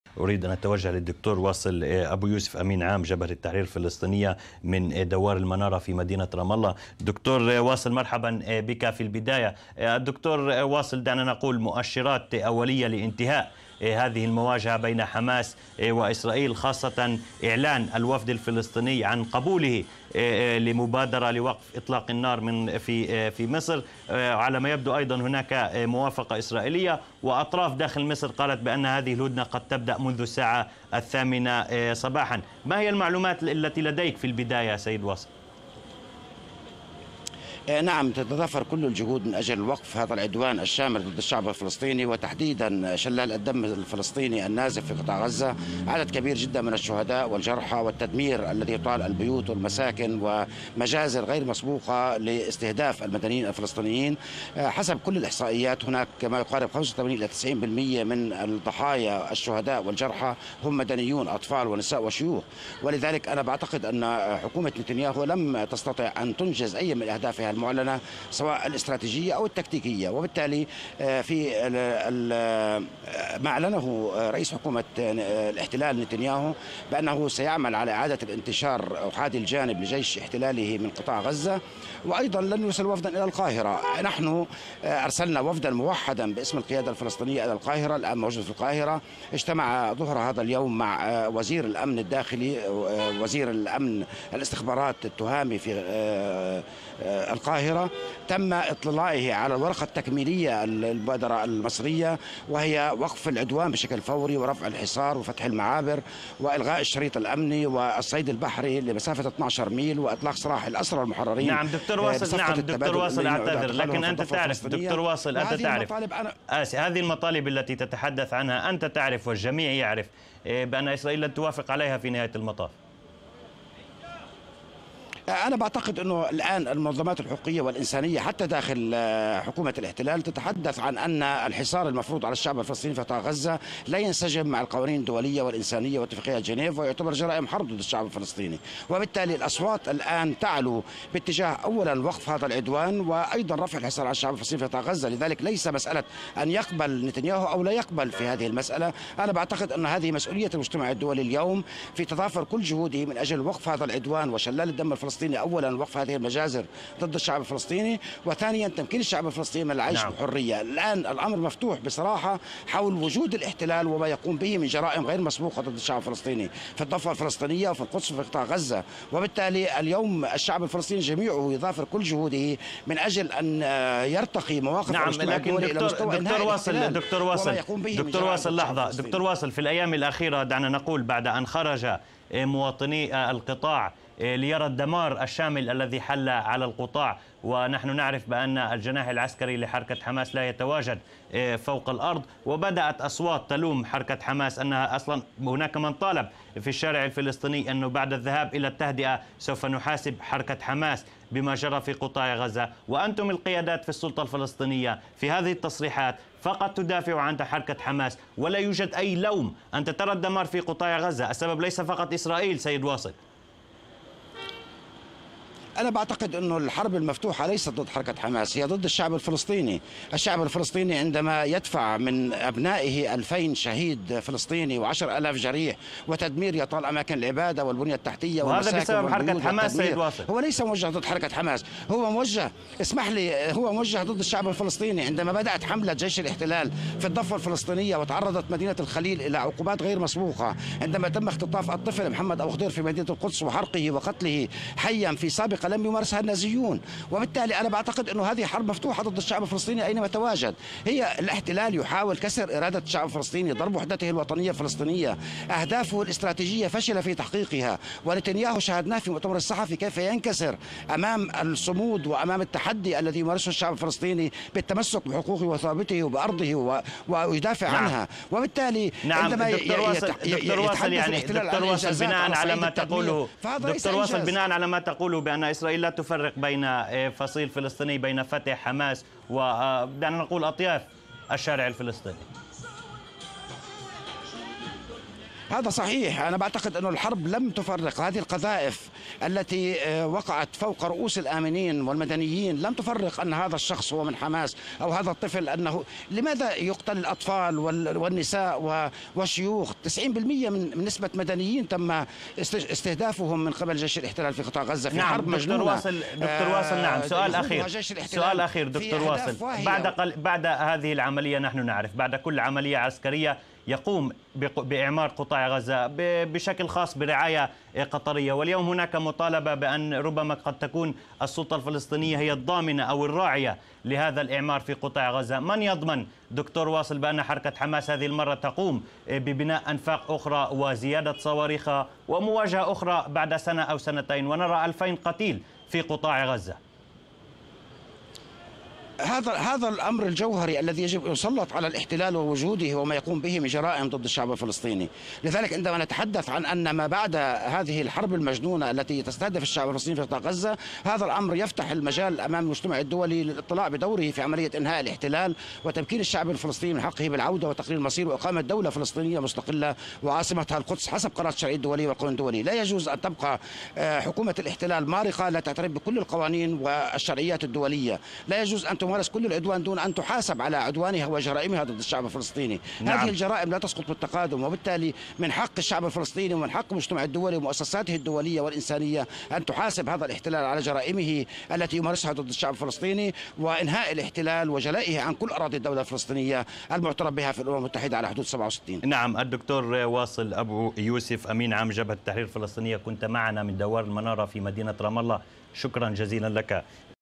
تغطية خاصة - 04/08/2014
مقابلة خاصة مع أمين عام جبهة التحرير الفلسطينية، الدكتور واصل أبو يوسف من رام الله، حول العملية العسكرية الإسرائيلية على قطاع غزة.